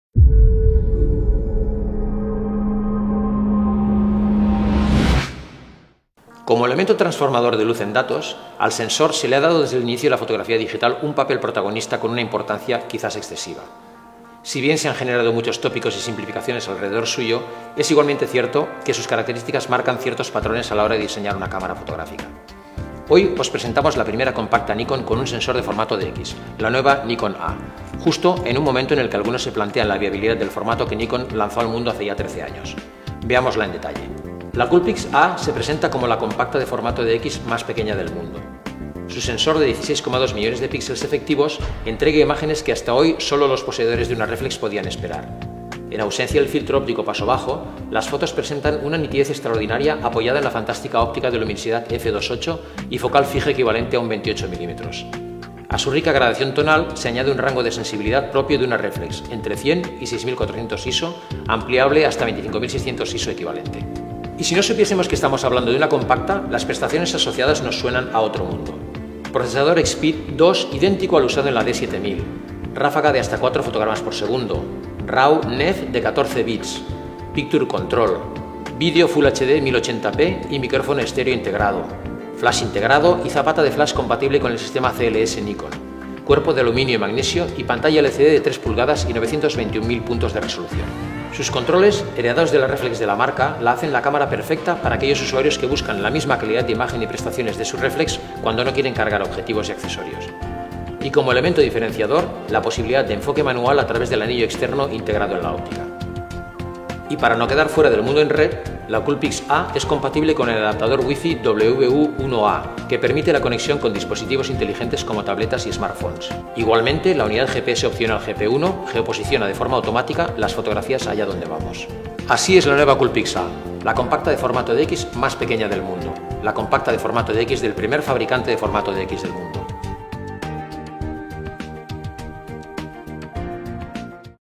Nikonistas TV: COOLPIX A
Equipada con un gran sensor CMOS de 16,2 megapíxeles de formato DX (23,6 x 15,6 mm) y un sistema de procesamiento de imágenes EXPEED 2, la COOLPIX A incluye tecnología DSLR en un formato compacto. El objetivo gran angular fijo NIKKOR de 18,5 mm f/2.8 (equivalente a 28 mm) ofrece una visión en alta definición desde el centro hasta las esquinas de la imagen, y un anillo de enfoque manual le permite cancelar el enfoque automático cuando le apetezca. La COOLPIX A también admite datos de imágenes RAW comprimidos de 14 bits (NEF), para que pueda ajustar en la misma cámara imágenes puras no procesadas.